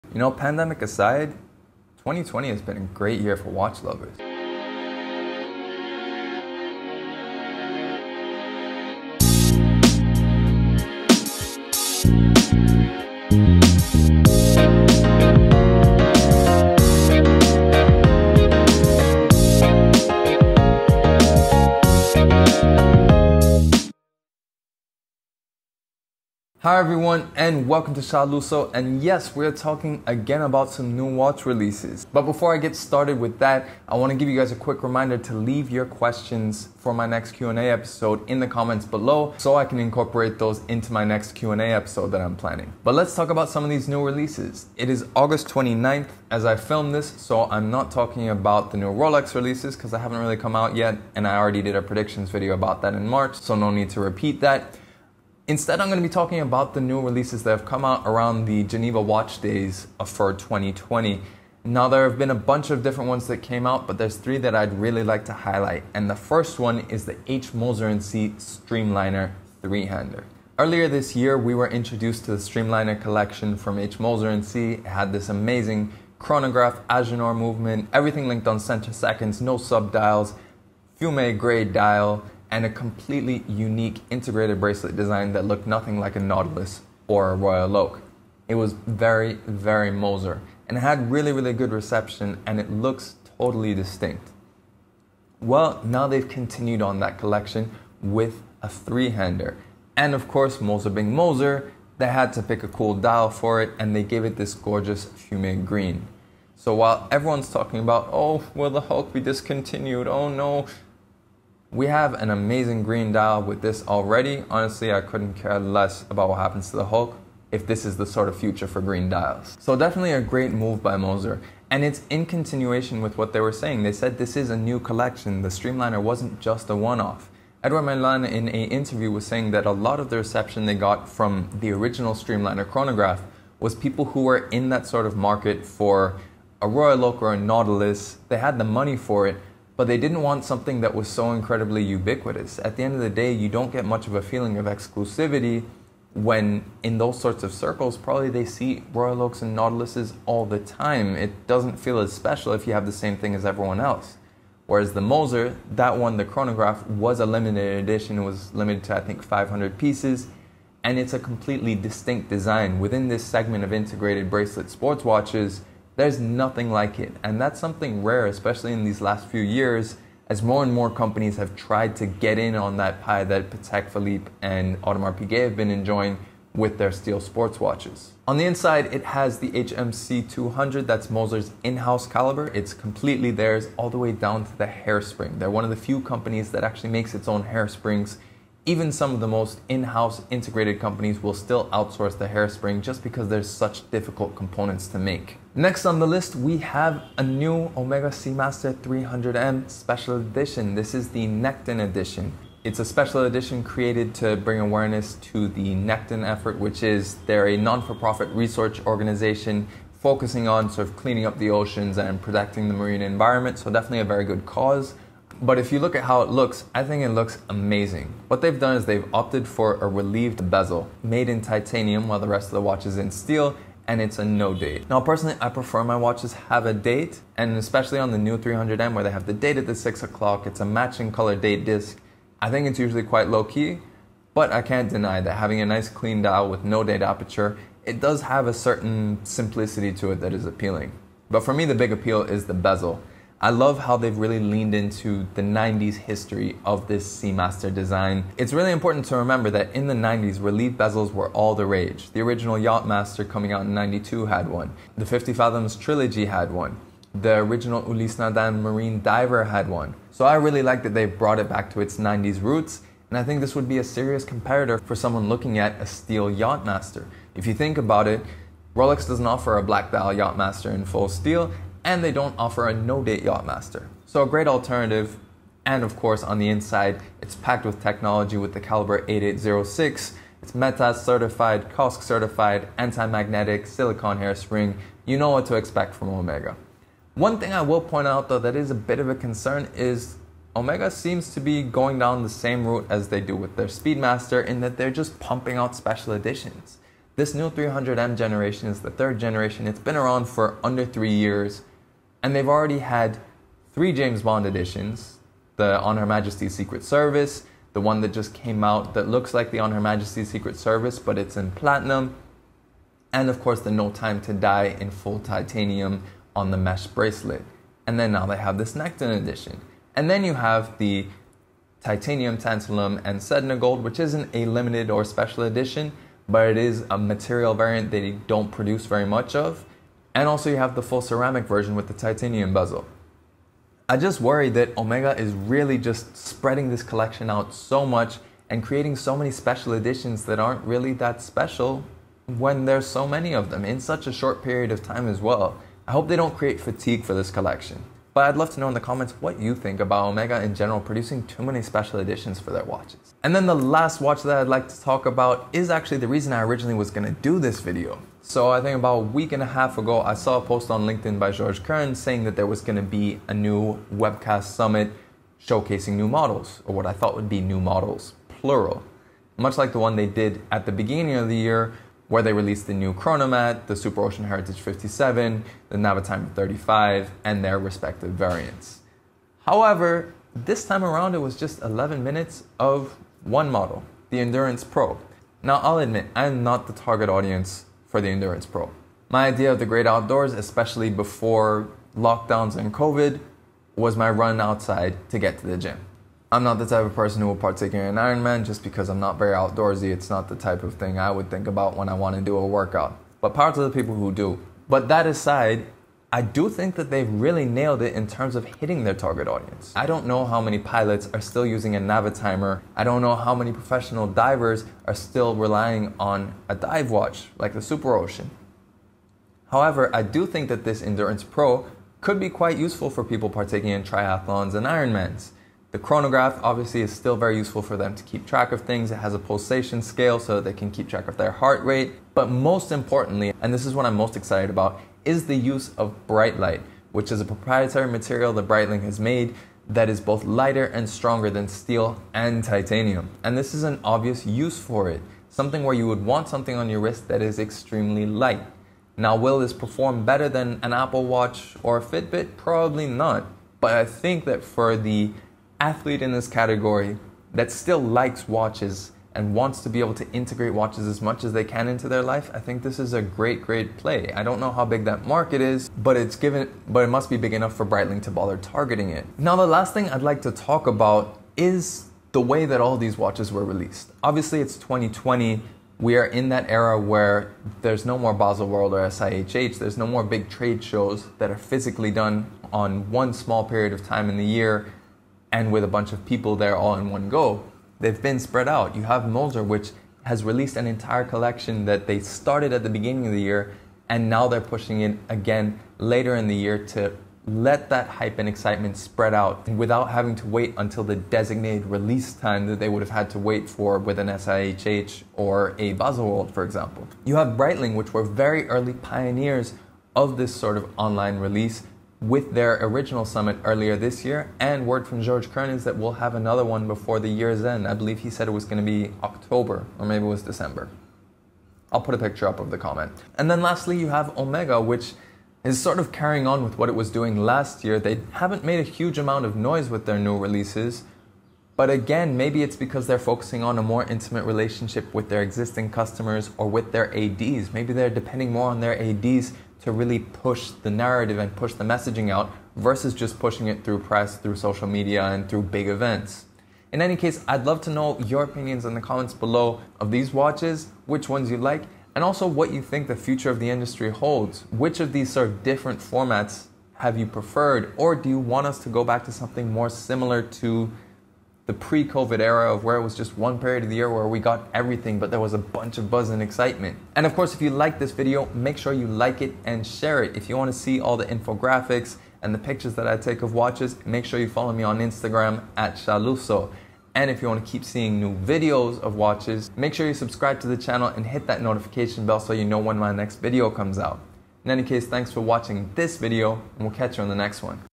2020 Omega Seamaster Nekton, Breitling Endurance, H Moser Streamliner Centre Seconds | New Releases
Today I’m discussing the latest batch of 2020 watch releases from Omega, Breitling & H Moser & Cie. At the end of August 2020 there were several new models released as part of Geneva Watch Days, but three models really stood out:

0:00 Intro
1:07 H Moser & Cie. Streamliner Centre Seconds (Ref: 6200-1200)
3:17 Omega Seamaster 300M Diver Nekton Edition (Ref: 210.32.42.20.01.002)
5:53 Breitling Endurance Pro (Ref: X82310D91B1S1)
7:49 The evolution of watch releases in 2020

What do you think of these new models from Breitling, Omega & H Moser? Which is your favorite & why? Where do you see the future of watch releases going in terms of timing & format? Which is your favorite new 2020 release so far?